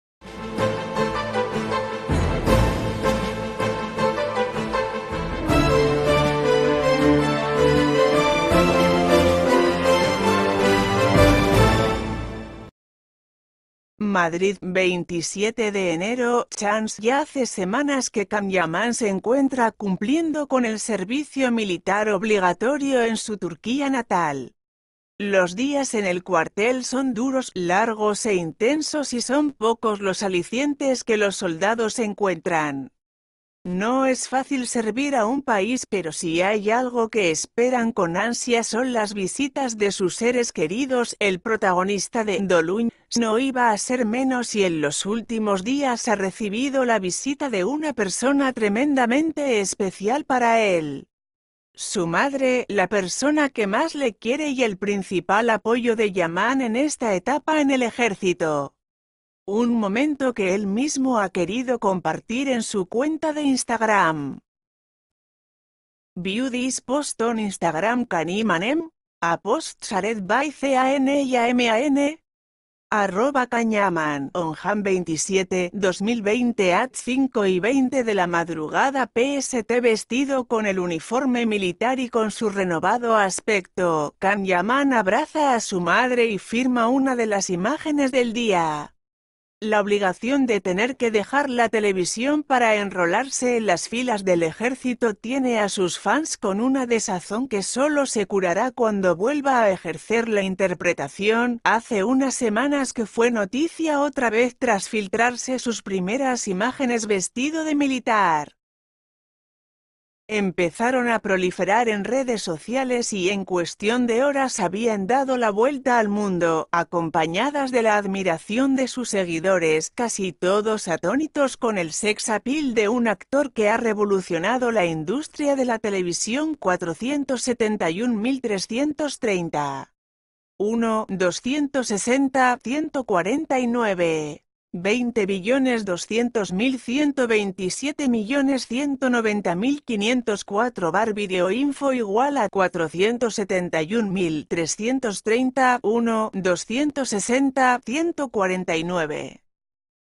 Can Yaman reaparece mientras cumple con el servicio militar en Turquía
MADRID, 27 Ene. (CHANCE) -    Ya hace semanas que Can Yaman se encuentra cumpliendo con el servicio militar obligatorio en su Turquía natal. Los días en el cuartel son duros, largos e intensos y son pocos los alicientes que los soldados encuentran. No es fácil servir a un país, pero si hay algo que esperan con ansia son las visitas de sus seres queridos.   El protagonista de 'Dolunay' no iba a ser menos y en los últimos días ha recibido la visita de una persona tremendamente especial para él: su madre; la persona que más le quiere y el principal apoyo de Yaman en esta etapa en el ejército. Un momento que él mismo ha querido compartir en su cuenta de Instagram. View this post on Instagram Canim annem ? A post shared by C a n Y a m a n ? (@canyaman) on Jan 27, 2020 at 5:20am PST   Vestido con el uniforme militar y con su renovado aspecto, Can Yaman abraza a su madre y firma una de las imágenes del día. La obligación de tener que dejar la televisión para enrolarse en las filas del ejército tiene a sus fans con una desazón que solo se curará cuando vuelva a ejercer la interpretación.   Hace unas semanas que fue noticia otra vez tras filtrarse sus primeras imágenes vestido de militar.